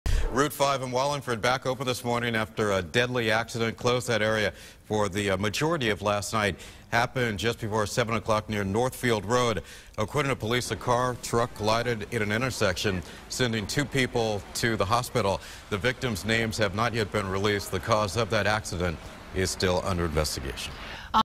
Wallingford PD: 1 dead, 2 transported to hospital after crash
Wallingford police are investigating after a crash kills one, sends two to the hospital Sunday night.